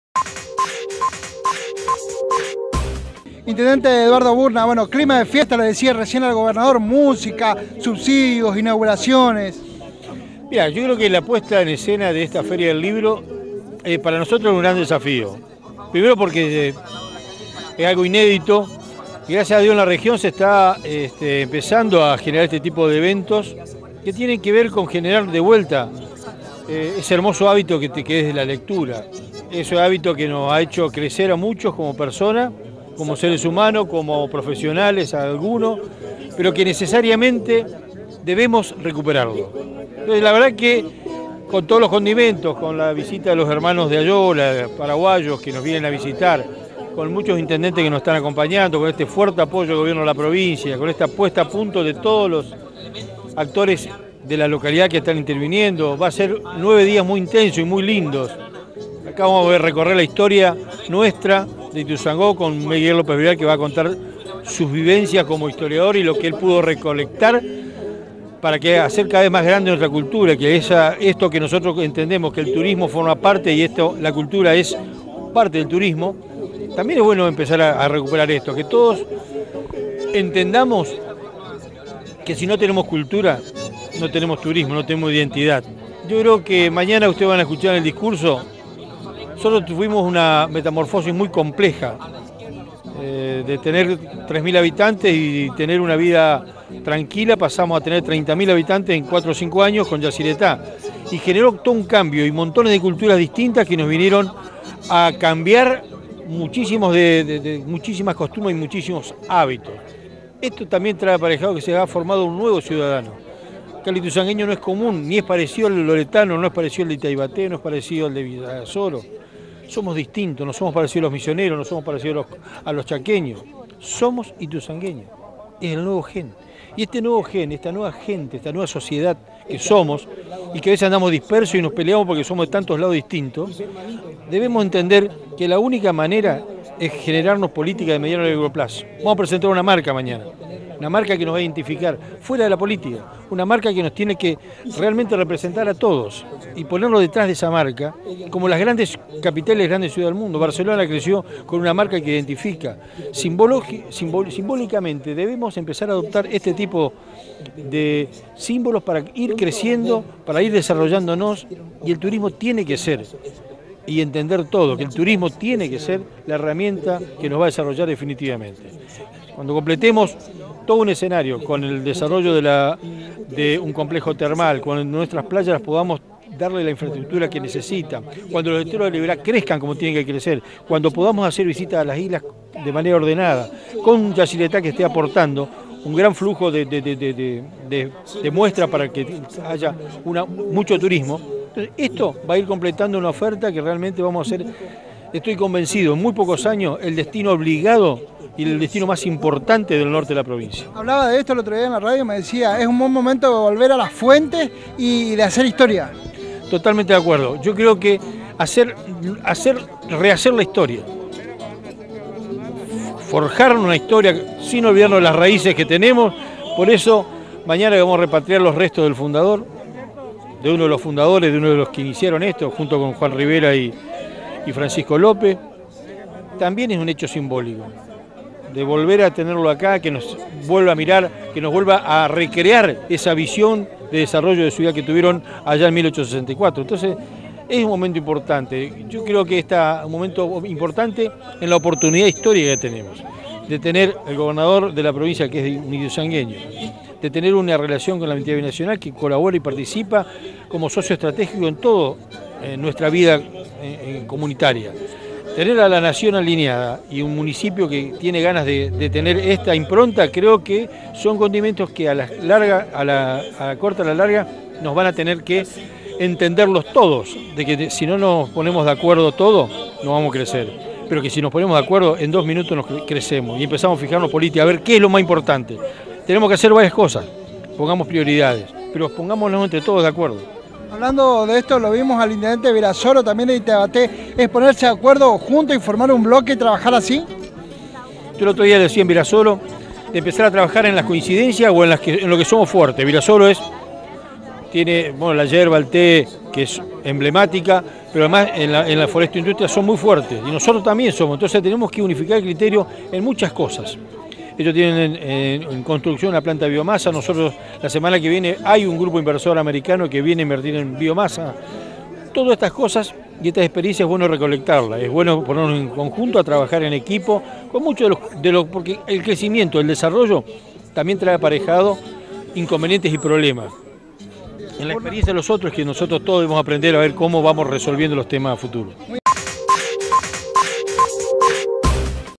El hábito de la Lectura en la 1ª Feria Regional del Libro
Eduardo Burna - Intendente